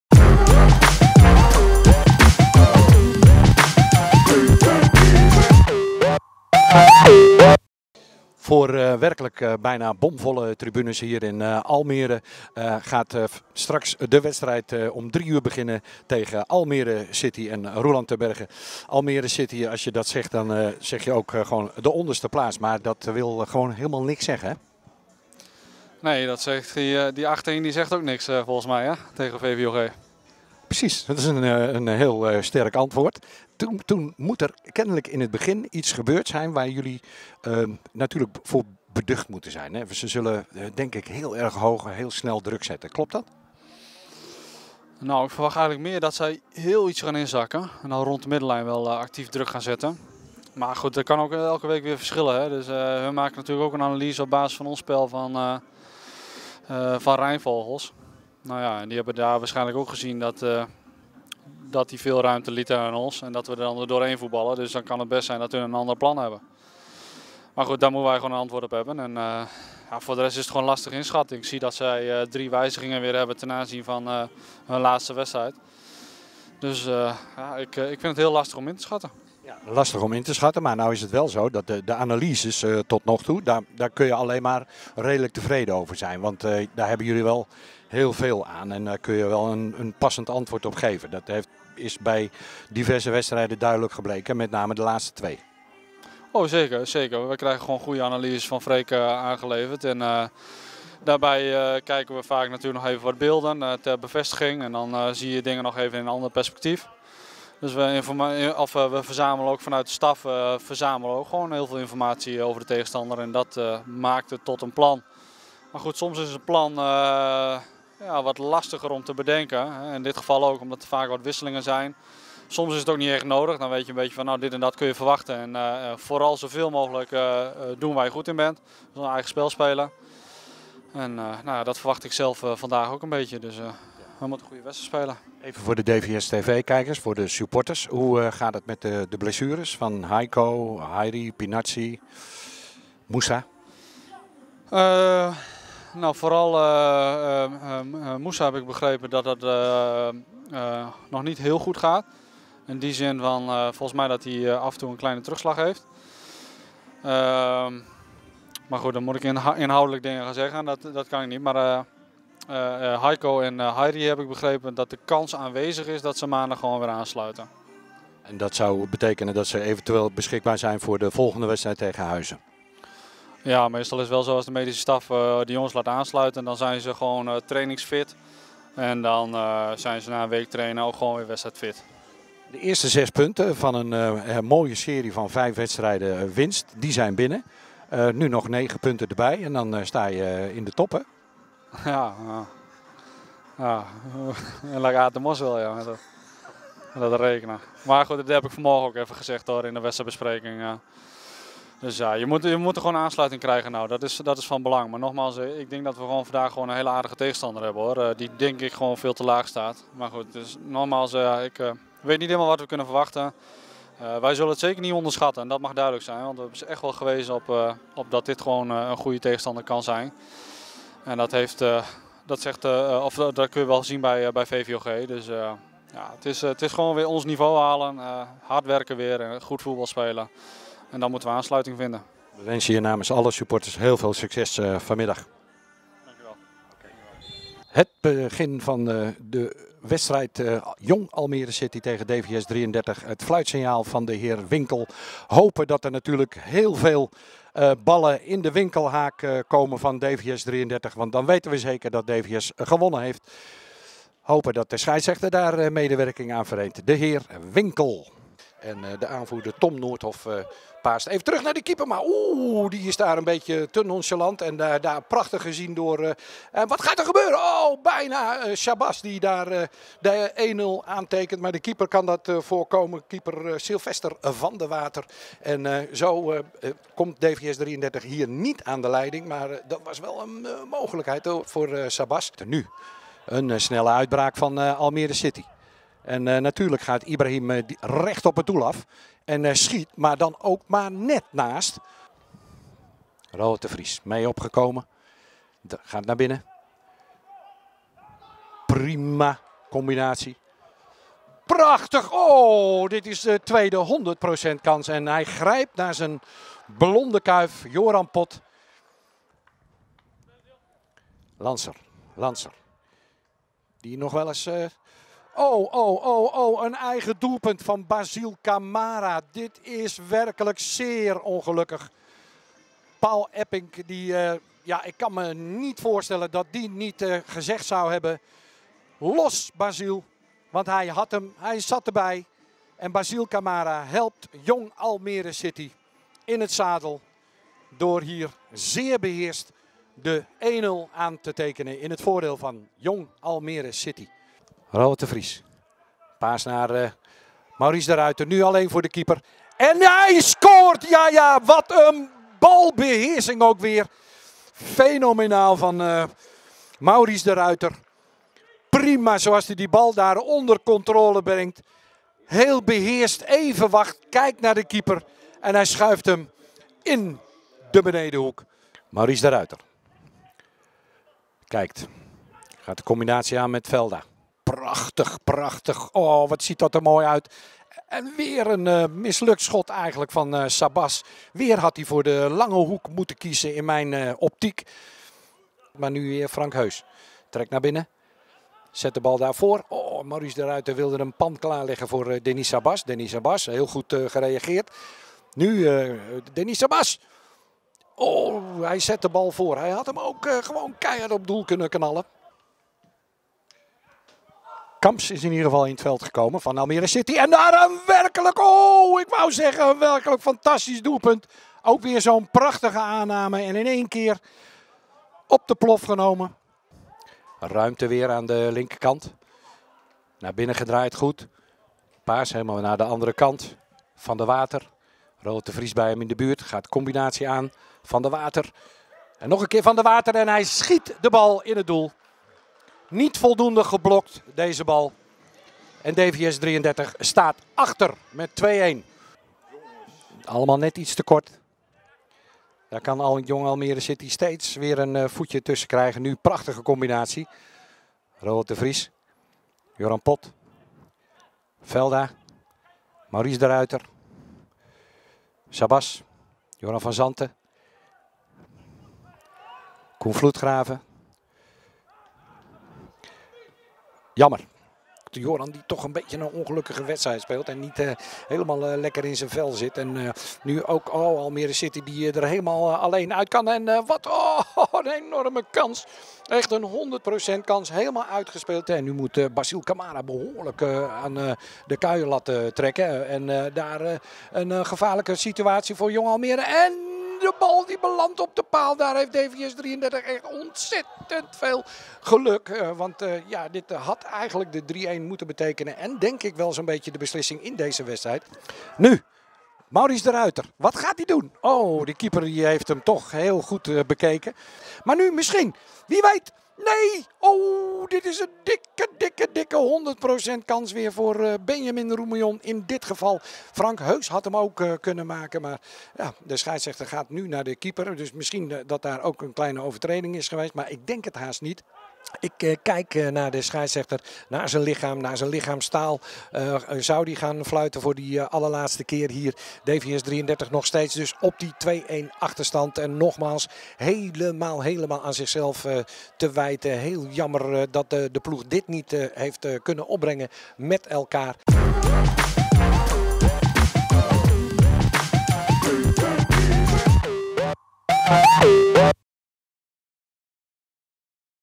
Jong Almere City FC - DVS'33 Ermelo 2-1
Op 1 oktober 2016 speelde DVS'33 Ermelo een uitwedstrijd tegen Jong Almere City FC, uitslag 2-1